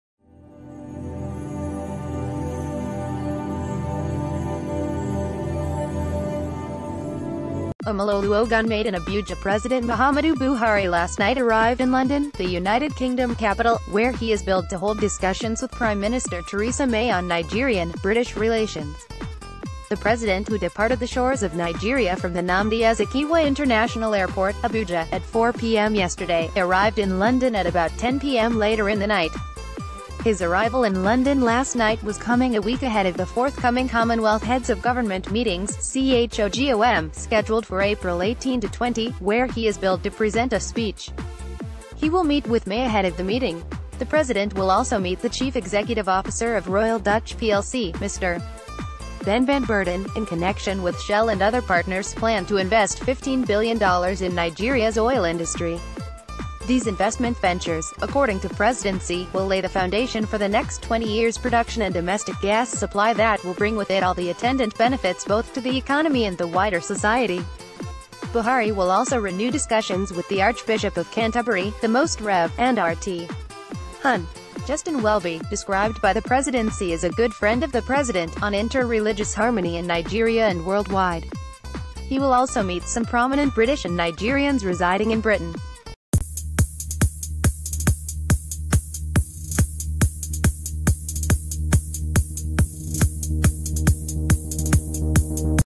Breaking News | Buhari Arrives London
Omololu Ogunmade in Abuja President Muhammadu Buhari last night arrived in London, the United Kingdom capital, where he is billed to hold discussions with Prime Minister Theresa May on Nigerian – British relations. The president who departed the shores of Nigeria from the Nnamdi Azikiwe International Airport, Abuja, at 4p.m. yesterday, arrived in London at about 10p.m. later in the night. His arrival in London last night was coming a week ahead of the forthcoming Commonwealth Heads of Government...